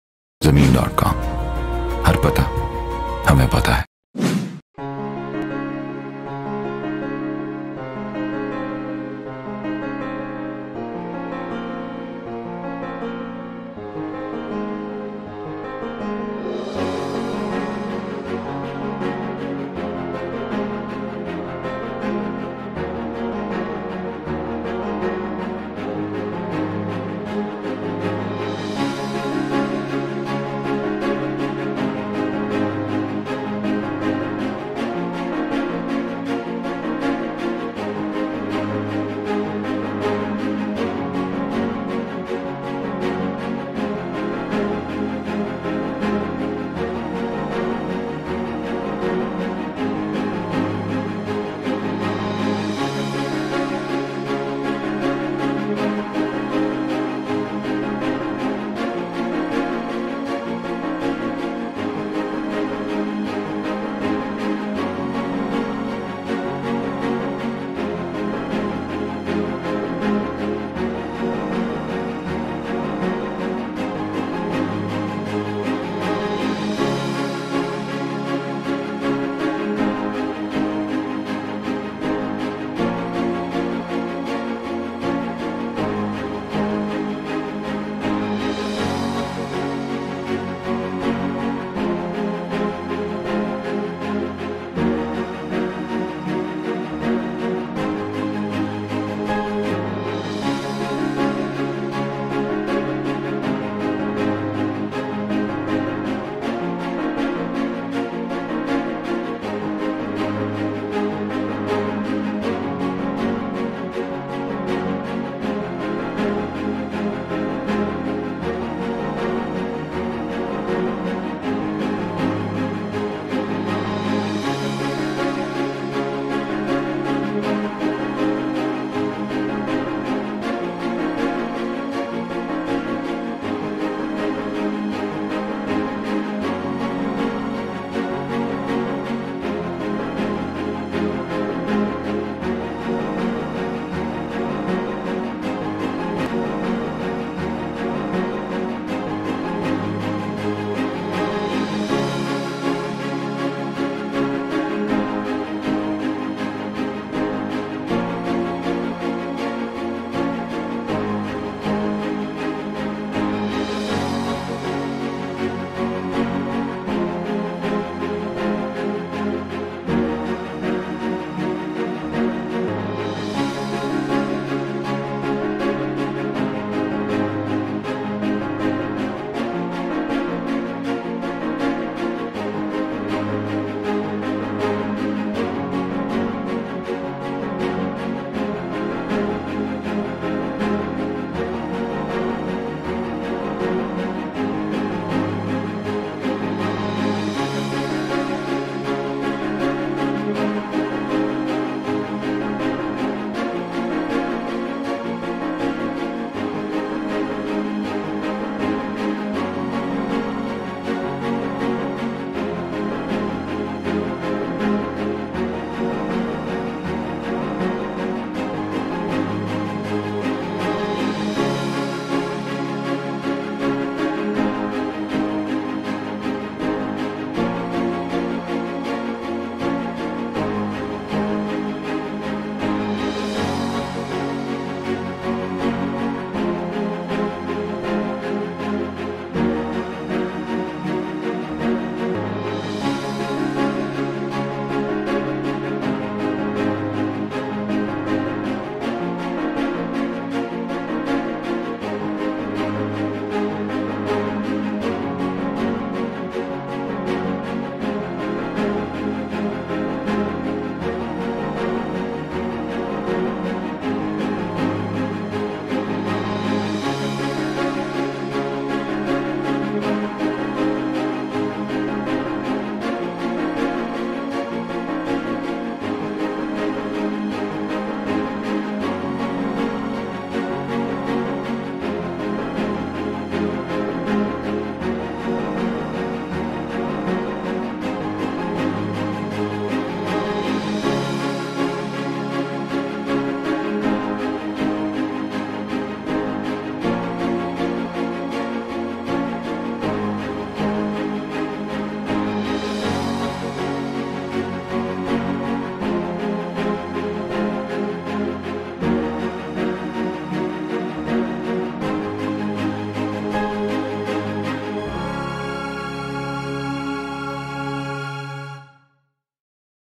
17.1 MARLA HOUSE FOR SALE IN SECTOR H ASKARI 5 MALIR CANTONMENT KARACHI
Location: Askari 5, Sector H, Malir Cantonment .
Having 5 Bed Rooms 5 Bath Rooms.
Ideal Location.
East Open.
Reasonable Demand.
Do not forget to mention Zameen. com while calling.